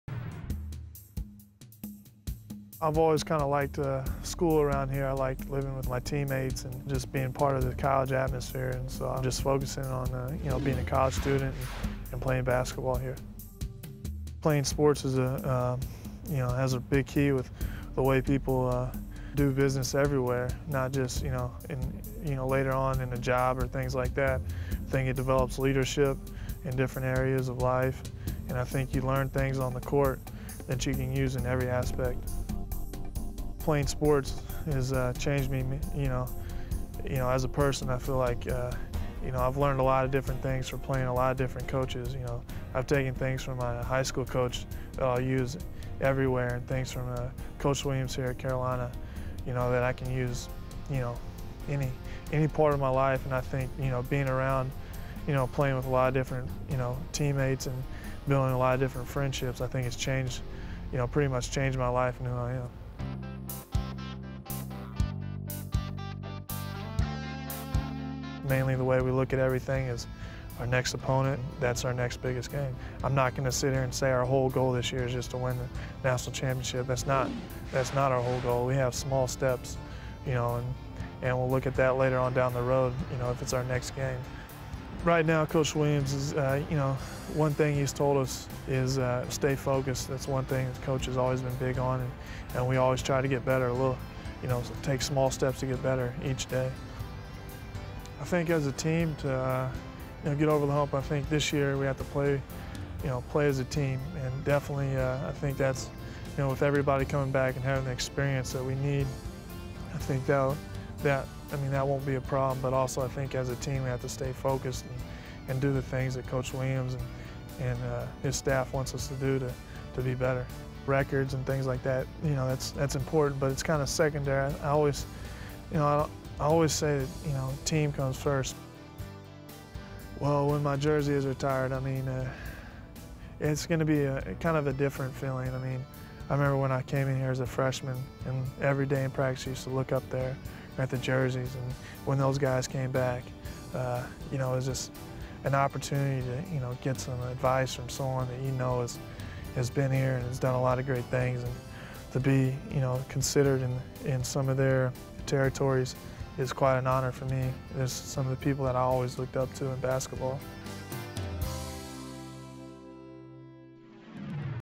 EA SPORTS College Basketball Special 2008 - Tyler Hansbrough
EA SPORTS visits reigning Player of the Year Tyler Hansbrough in Chapel Hill